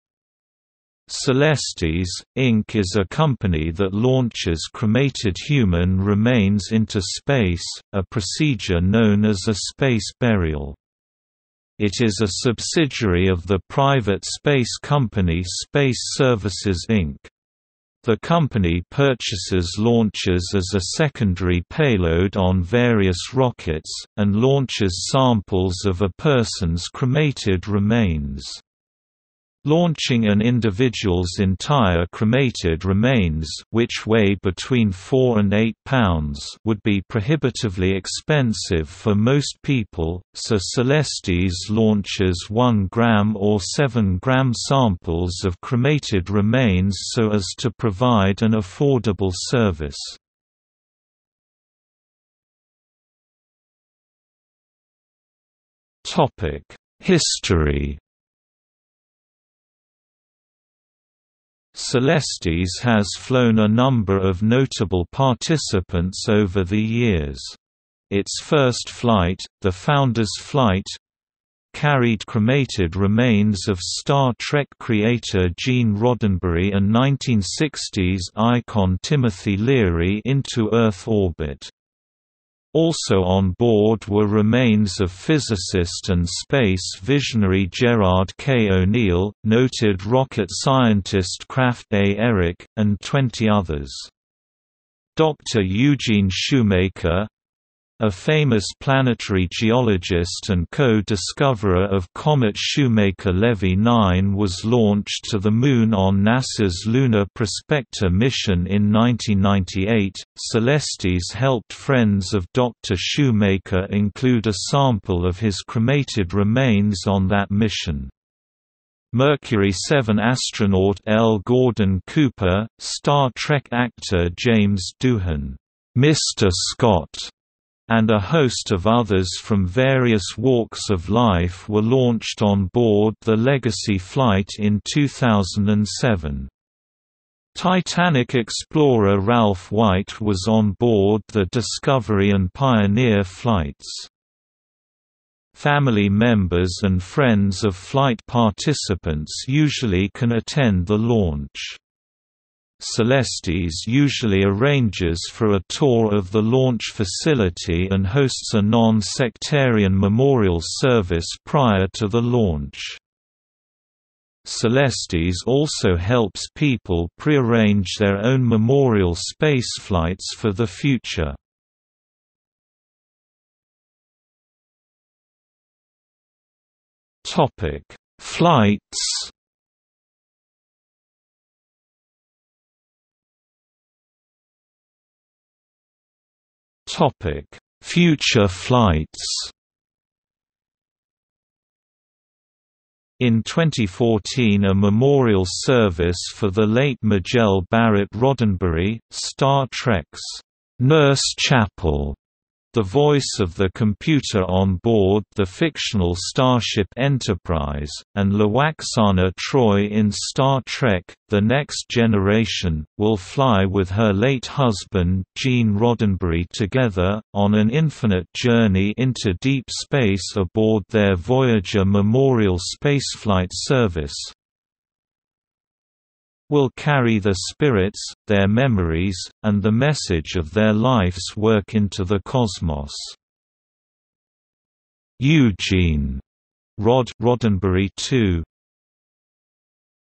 Celestis | Wikipedia audio article
00:00:52 1 History
00:02:53 2 Flights
00:03:03 3 Future flights

- Socrates

SUMMARY
Celestis, Inc. is a company that launches cremated human remains into space, a procedure known as a space burial. It is a subsidiary of the private space company Space Services Inc. The company purchases launches as a secondary payload on various rockets, and launches samples of a person's cremated remains. Launching an individual's entire cremated remains (which weigh between four and eight pounds) would be prohibitively expensive for most people, so Celestis launches 1 gram or 7 gram samples of cremated remains so as to provide an affordable service.